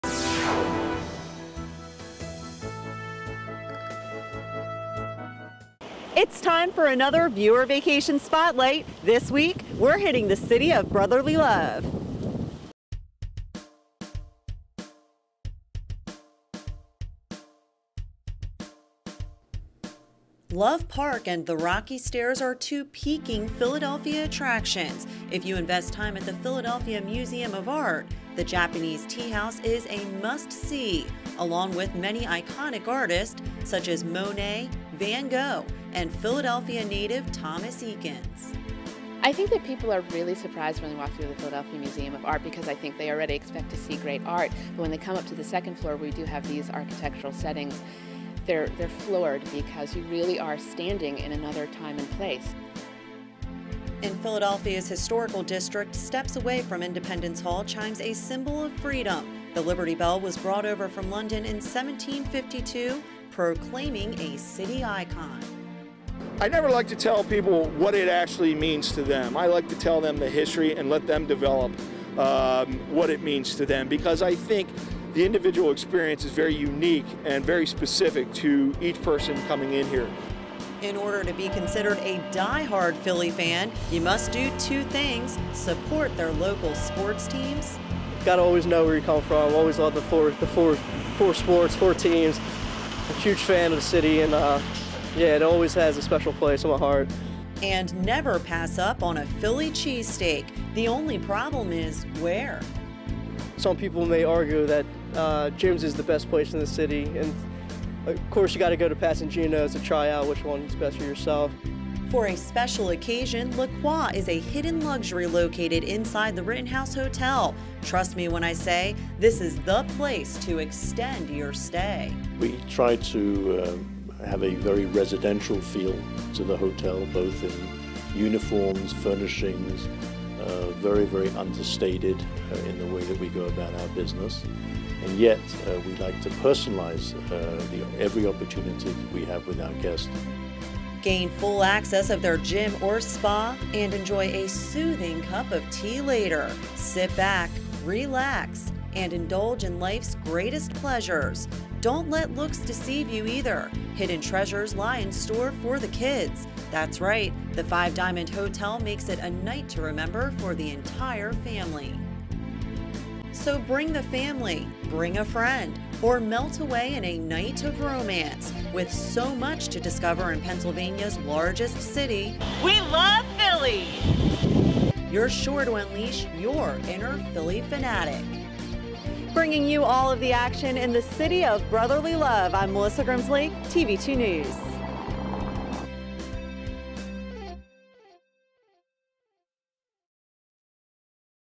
Philadelphia.mov
The final Viewer Vacation segment may not have made it to air, but our city is still leaving an impact. Enjoy a glimpse of the Philadelphia Art Museum, Liberty Bell, and Rittenhouse Hotel; all in the heart of Philly.